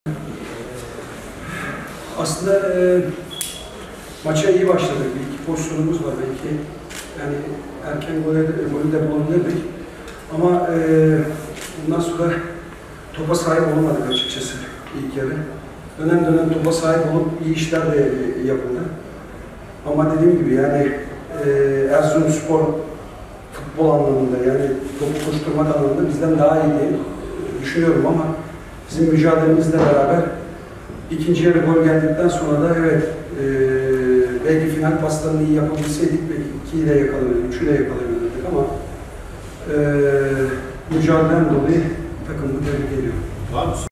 Tütüneker;Takımı tebrik ediyorum
TFF 1.Lig ekiplerinden Adana Demirspor’un Teknik Direktörü Uğur Tütüneker, 1-0 kazandıkları BB Erzurumspor maçı sonrası, “Final paslarını iyi yapabilseydik iki, üçü de yakalayabilirdik ama mücadele eden takımı tebrik ediyorum” dedi.
“İyi işler de yapıldı”
Tütüneker, maç sonu gazetecilere yaptığı açıklamada, “Aslında maça iyi başladık bir iki pozisyonunuz vardı. Erken golü de bulabilirdik ama ondan sonra topa sahip olamadık açıkçası ilk yarı. Dönem dönem topa sahip olup iyi işler de yapıldı ama dediğim gibi Erzurumspor’un futbol anlamında, topu koşturmak anlamında bizden daha iyi olduğunu düşünüyorum. Bizim mücadelemizle beraber ikinci yarı gol geldikten sonra da final paslarını iyi yapabilseydik iki, üçü de yakalayabilirdik ama mücadele eden takımı tebrik ediyorum” diye konuştu.